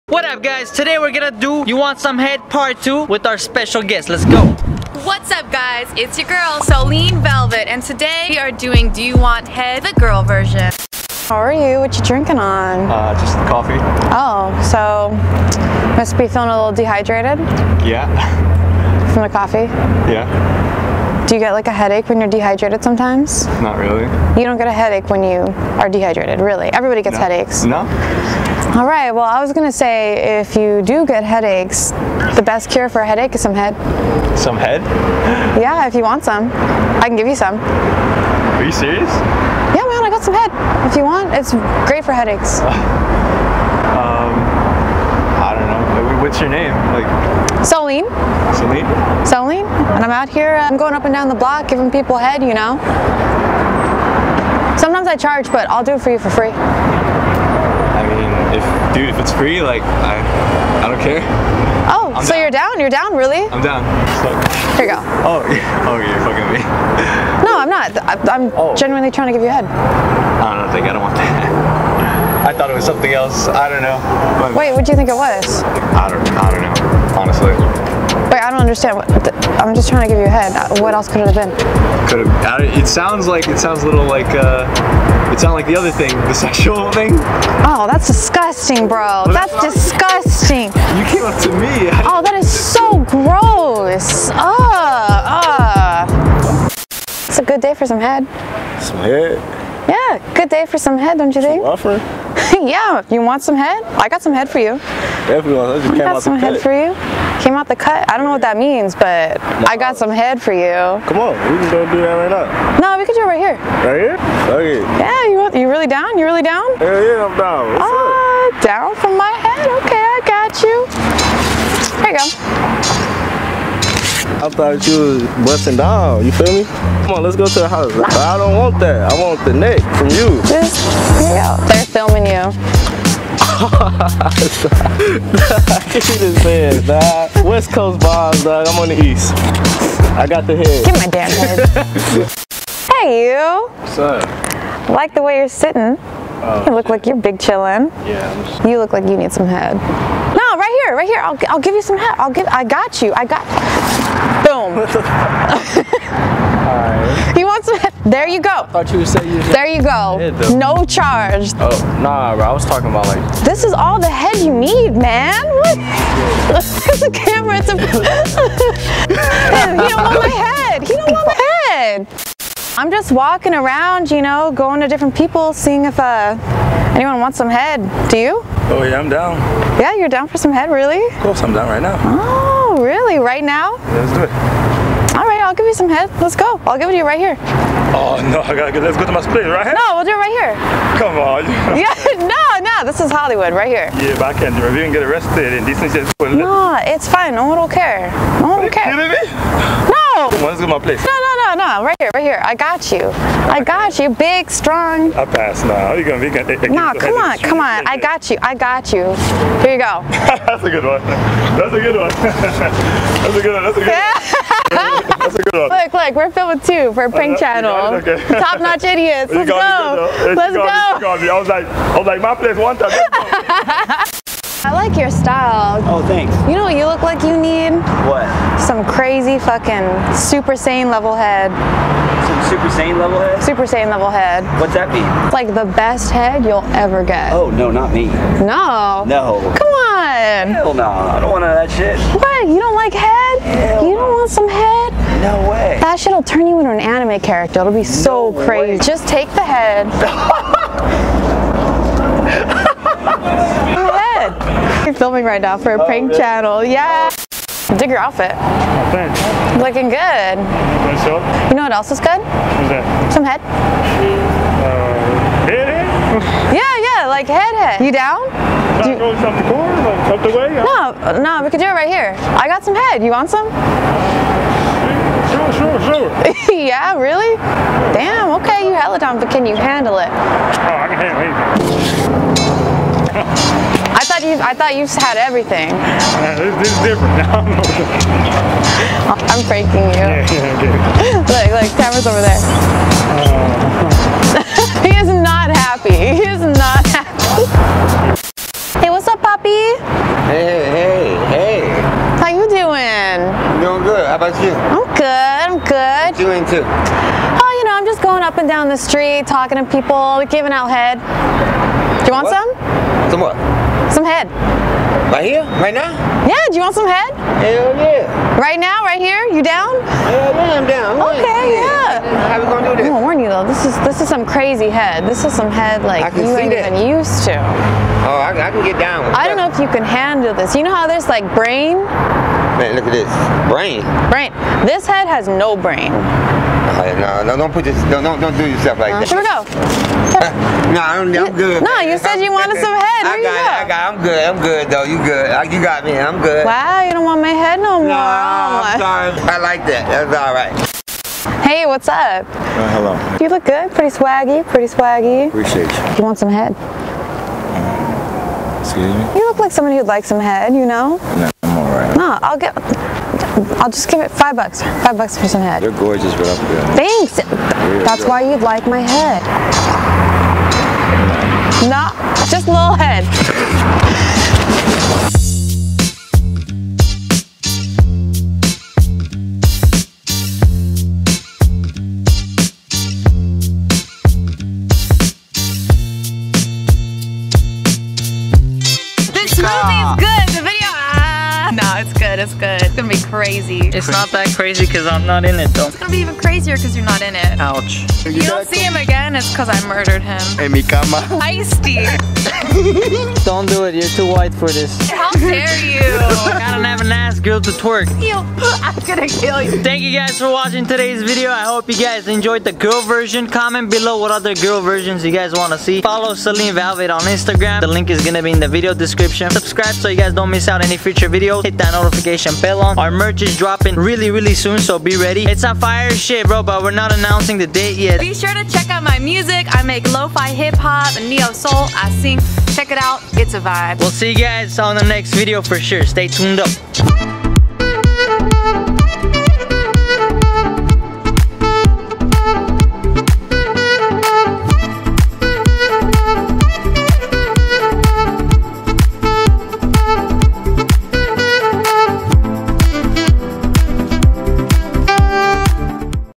Can I Give You Head?
CAN I GIVE YOU SOME HEAD PRANK IN THE HOOD! (GIRL VERSION)
MUST WATCH THIS FUNNY PRANK VIDEO

We make all kinds of stupid videos to make you smile more! From gold digger pranks to hood videos to social experiments in public!

FOLLOW SOLEN ON SOCIAL MEDIA!

One Love to All Of You!